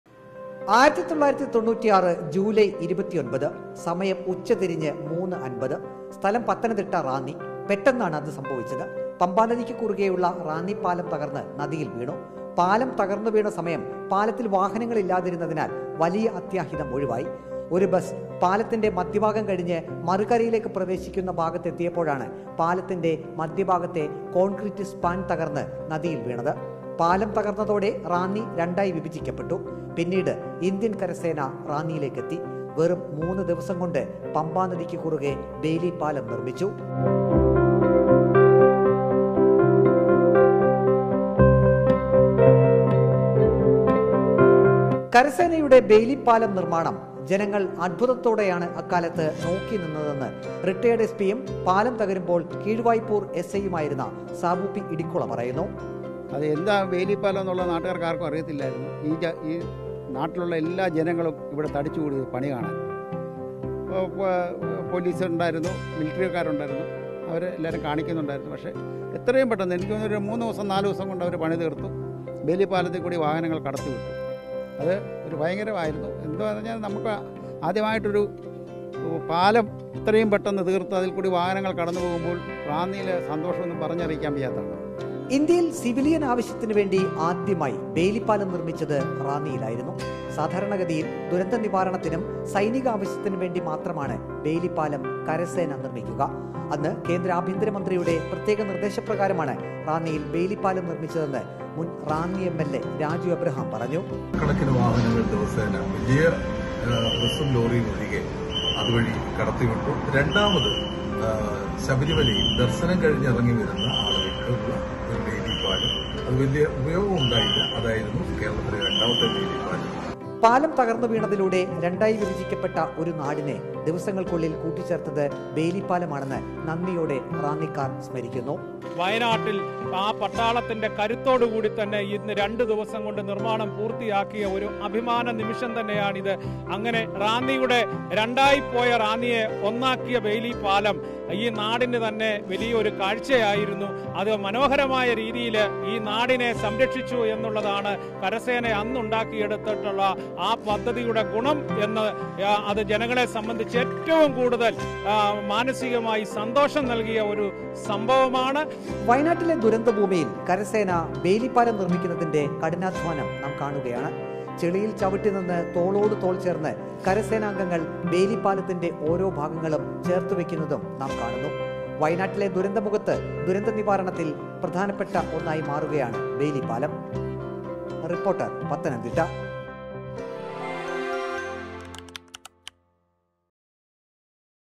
'രണ്ടായിപ്പോയ ഒരു നാടിനെ ദിവസങ്ങള്‍ക്കുള്ളില്‍ കൂട്ടിച്ചേര്‍ത്തത് ബെയ്‌ലി പാലമാണ്' | Bailey Bridge
'രണ്ടായി വിഭജിക്കപ്പെട്ട ഒരു നാടിനെ ദിവസങ്ങള്‍ക്കുള്ളില്‍ കൂട്ടിച്ചേര്‍ത്തത് ബെയ്‌ലി പാലം ആണ്', നന്ദിയോടെ റാന്നിക്കാര്‍ സ്മരിക്കുന്നു | Bailey Bridge | Pathanamthitta | Ranni

With Regards
Team RBC